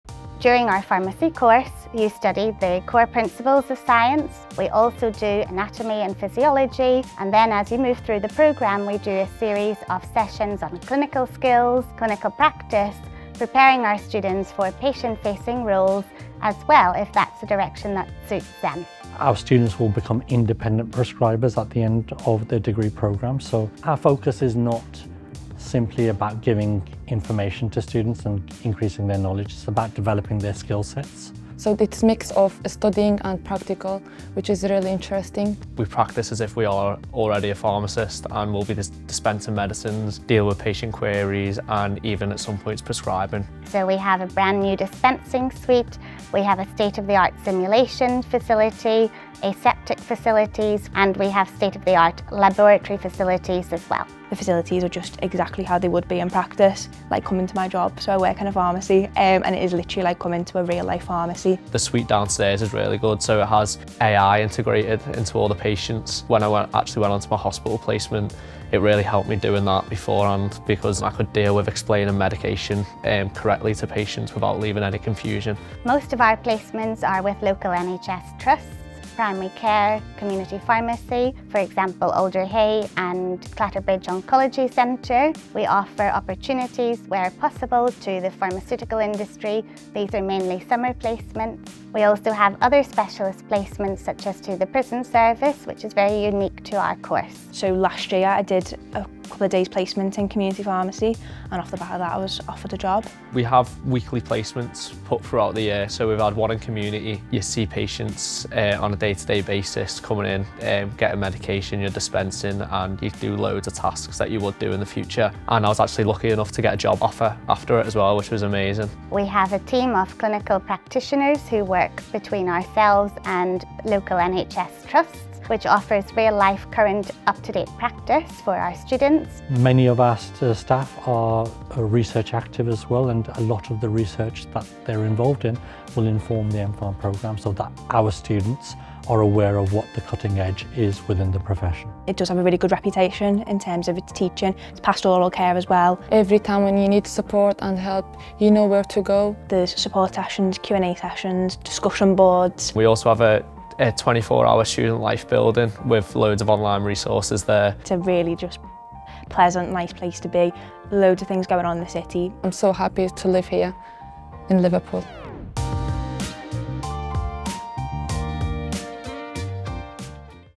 MPharm Pharmacy at Liverpool John Moores University
The professionally-accredited MPharm Pharmacy features innovative and high-quality pharmacy teaching, plus opportunities to complete placements with leading local hospitals, community pharmacies and primary care teams.